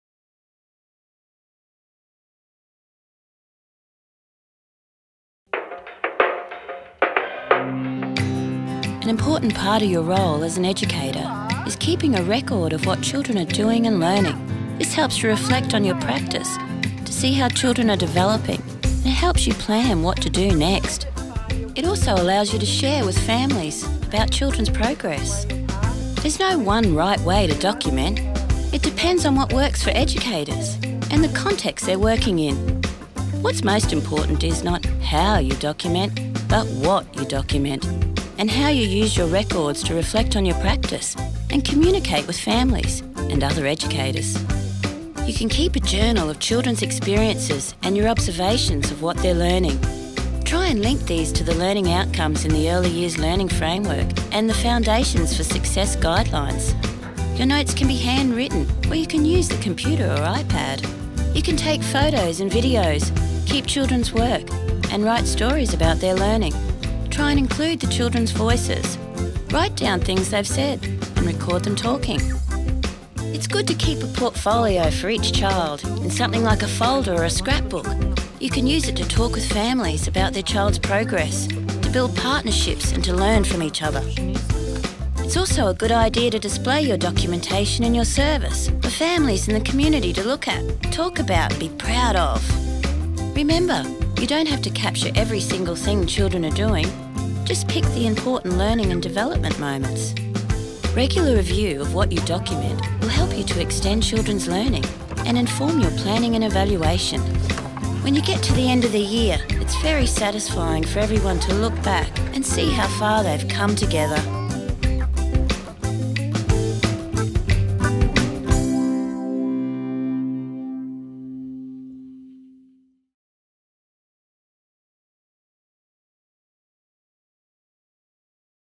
Documenting and Reflecting
Educators share how documenting and reflecting is an important part of your role, keeping a record of what children are doing and learning. Displaying documentation for families and the community to look at, talk about, and be proud of. To learn more from educators reflecting into their early years practice,